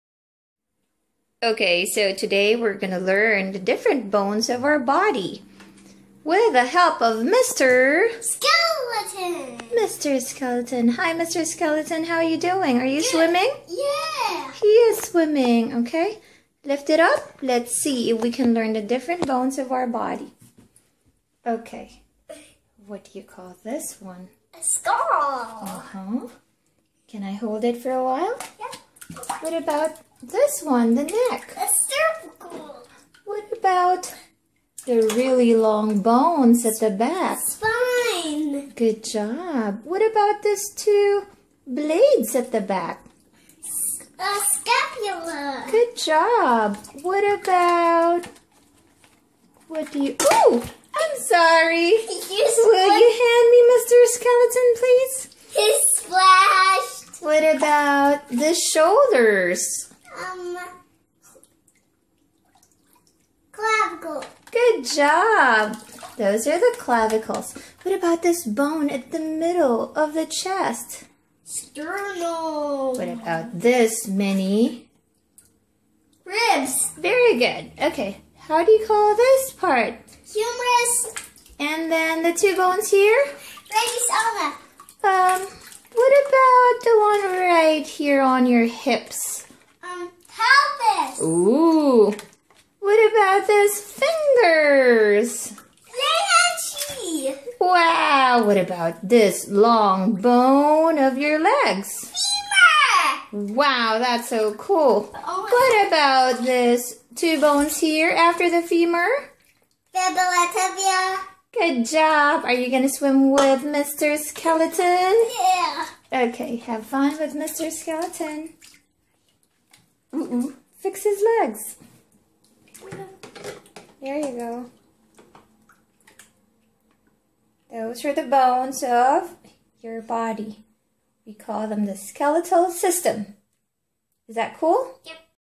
Learn the Bones of Your Body Skeletal System from a 4-Year Old Smart Kid featuring Mr. Skeleton
Learn the Skeletal System from a smart kid.